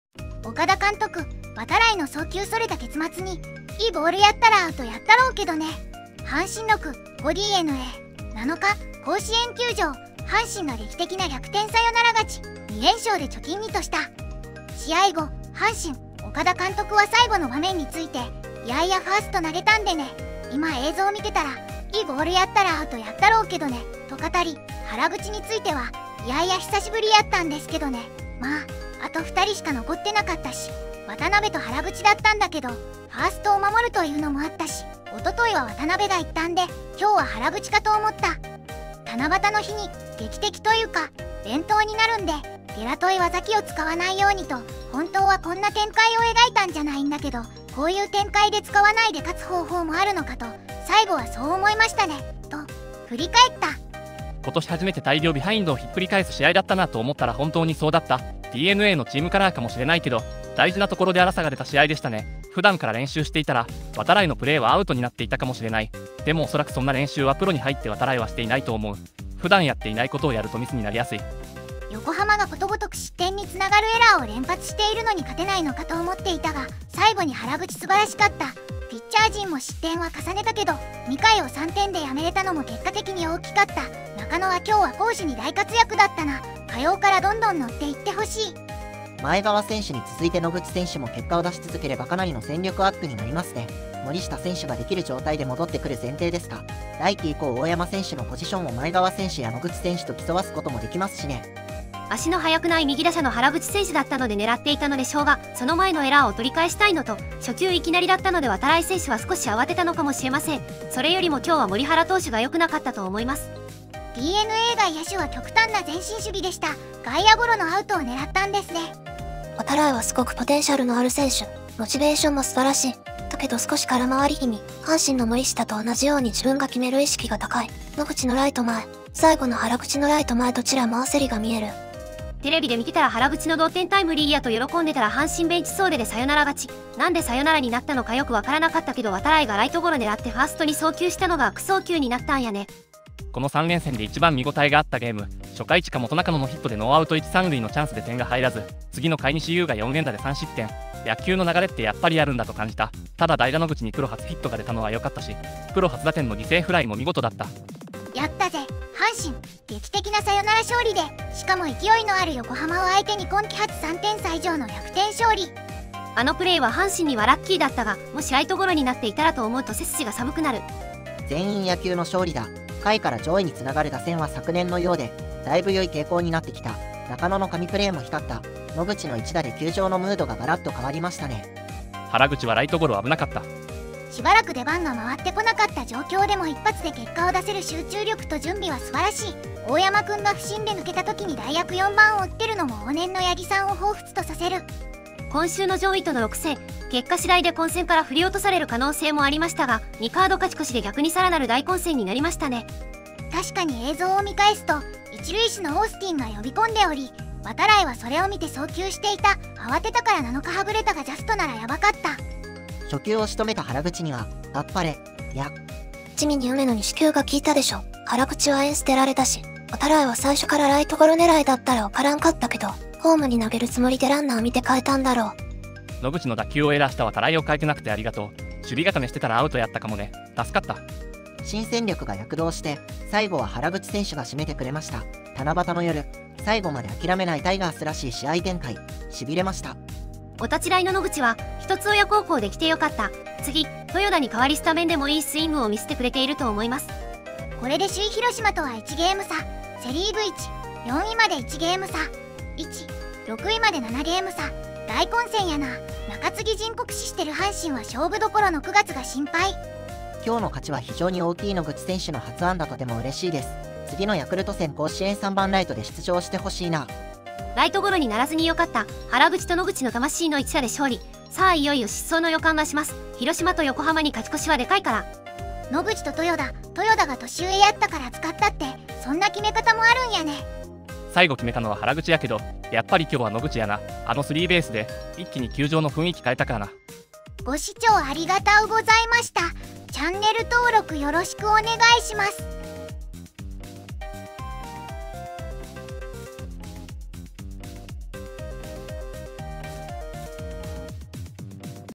阪神・岡田監督、度会の送球それた結末に「いいボールやったらアウトやったろうけどね」 【ネットの反応】【反応集】
VOICEVOX:ずんだもん
VOICEVOX:四国めたん
VOICEVOX:玄野武宏
VOICEVOX:白上虎太郎
VOICEVOX:ナースロボ＿タイプＴ

引用元　ttps://news.yahoo.co.jp/articles/f48fb5c0b6c5044a599a1b71ef32fb5982b829a7